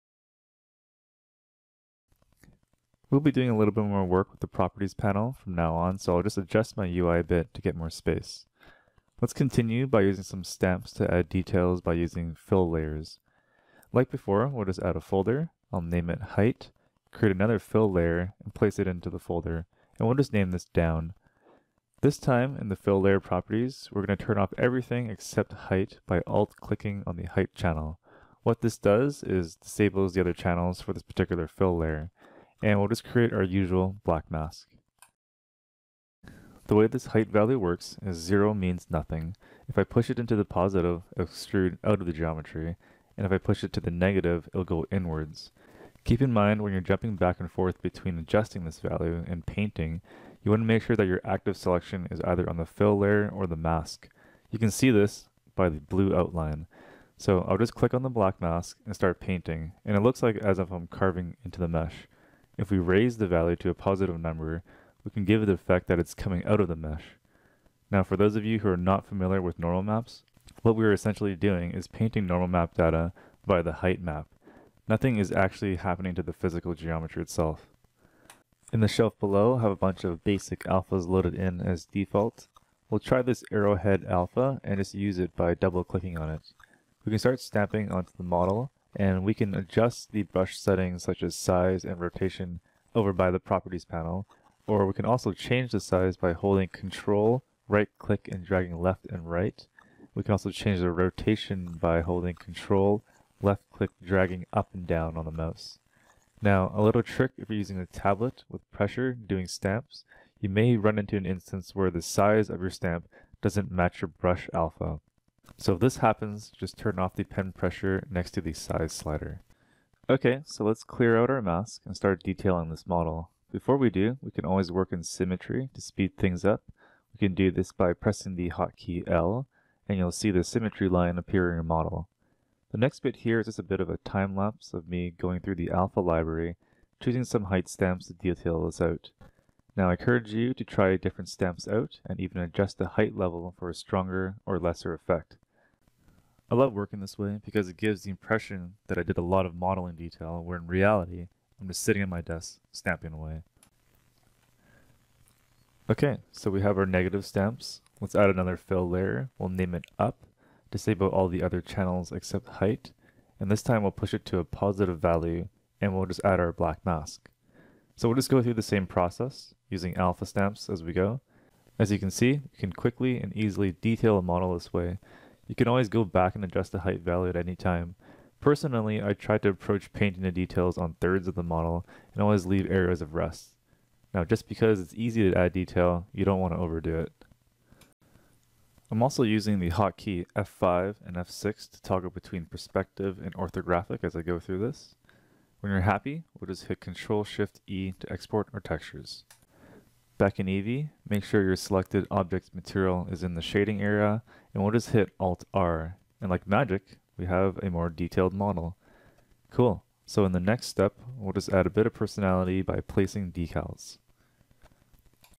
EEVEE + SP - 03 Height Stamps
Lesson 03 - In this video we will start stamping normal map data using height fill layers.  This is a great way to add detail quickly to your textureset.